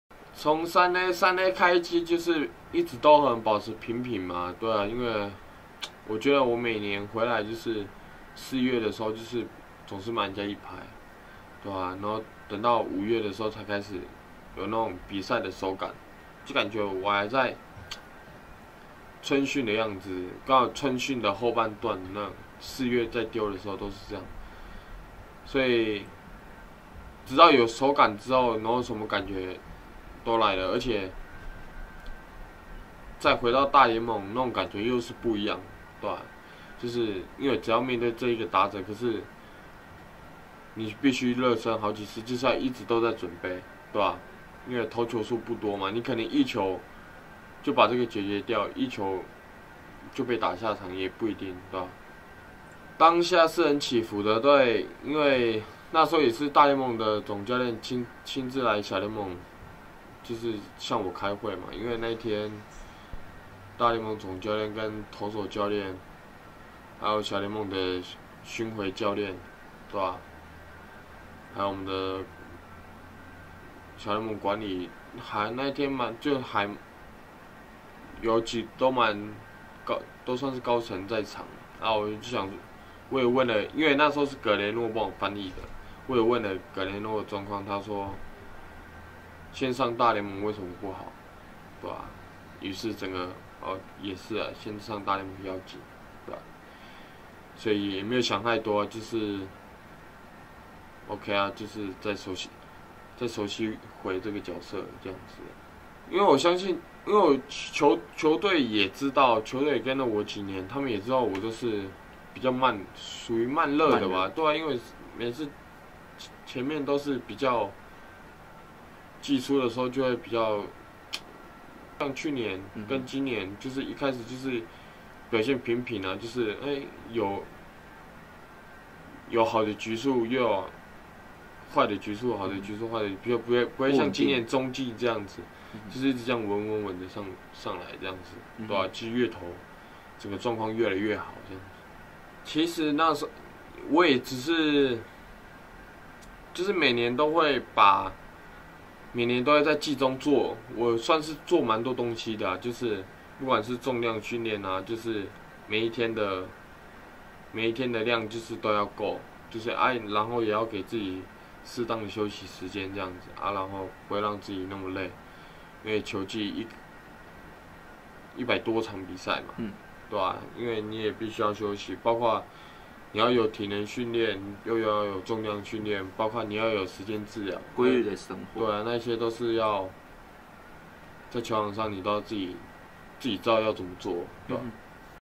身為慢熱型投手 王維中坦言轉中繼時心情起伏
今年春訓後被告知3A將擔任中繼的心情是如何，王維中指出：「是有起伏，因為當時大聯盟總教練親自來開會，然後跟我說今年要轉中繼，當時幫我翻譯的是葛雷諾，我問一下他，他說能上大聯盟為什麼不好？我也認為先上大聯盟要緊，所以也沒想太多，就再熟悉回這個角色。」

針對3A開季不穩後如何把狀況調整回來，尤其是後段的表現真的很好，王維中說：「我每年季中都會做滿多東西的，不管是重量訓練還是什麼，每天的量都要做到夠，然後有適當的休息，不讓自己那麼累，球季一百多場比賽你必須要休息，期間有體訓練、重量訓練，還要有時間治療，這些你都要知道自己該怎麼做。」

【未經同意請勿使用、翻拍、下載或嵌入】